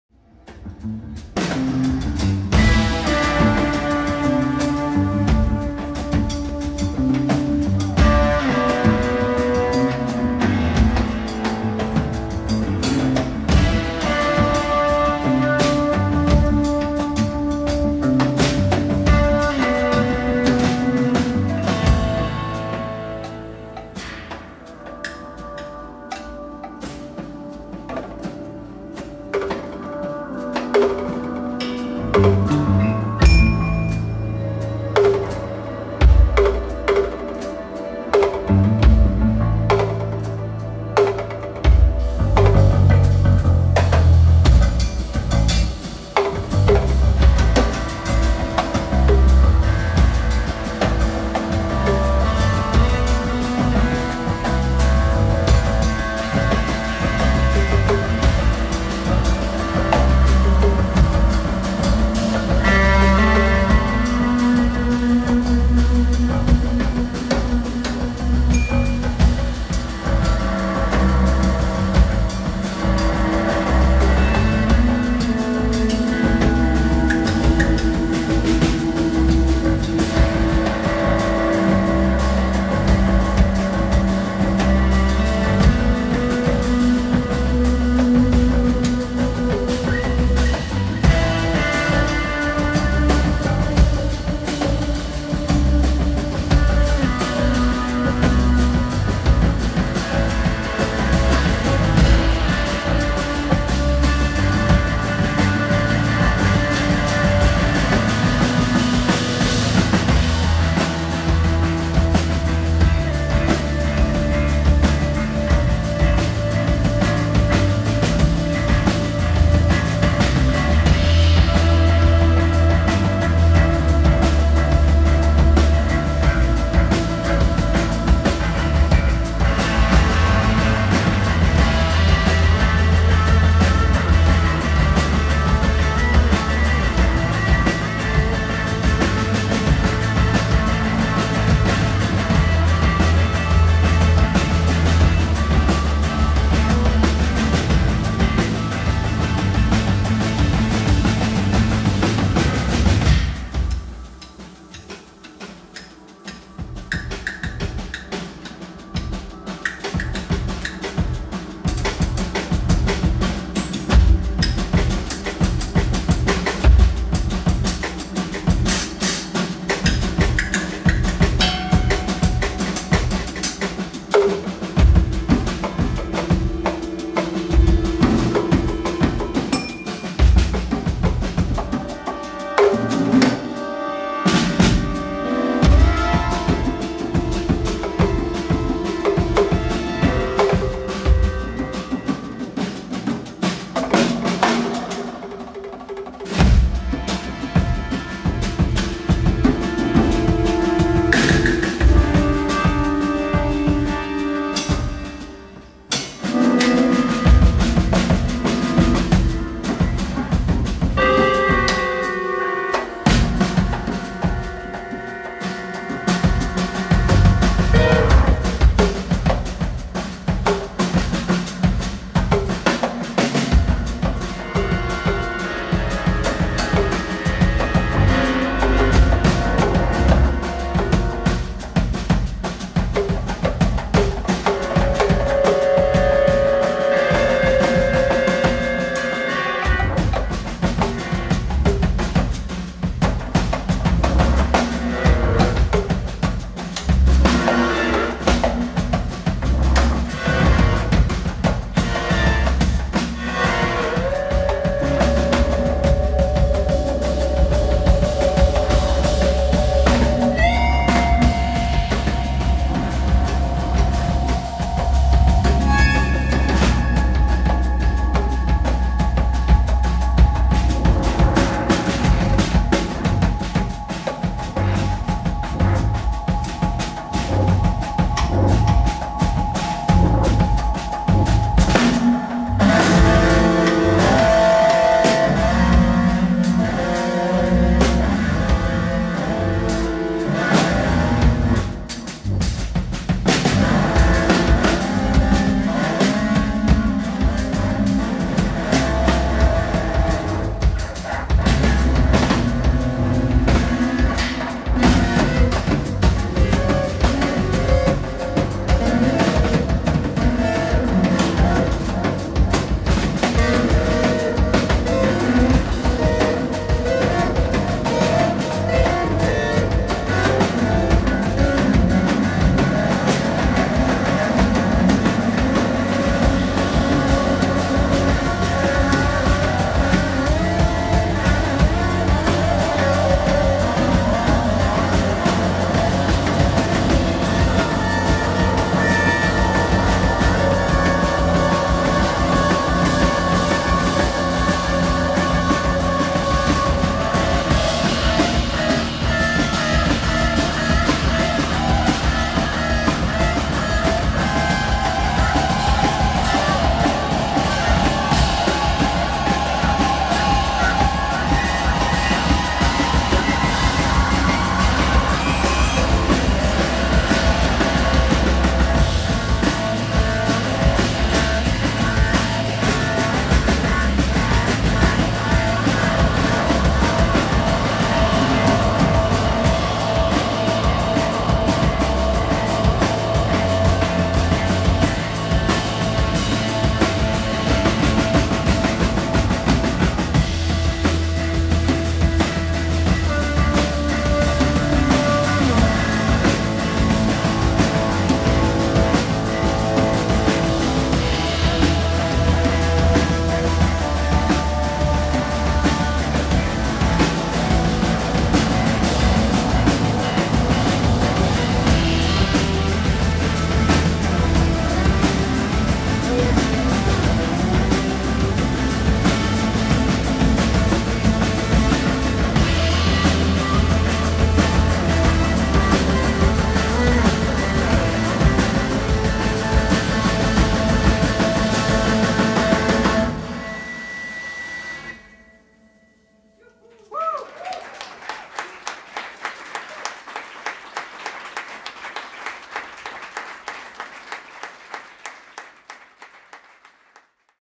EIVIND AARSET | Bergen Jazzforum
SARDINEN USF: 19. mars 21 Composed by EIVIND AARSET guitar
AUDUN ERLIEN bass - ERLAND DAHLEN drums, perc.
WETLE HOLTE drums, perc -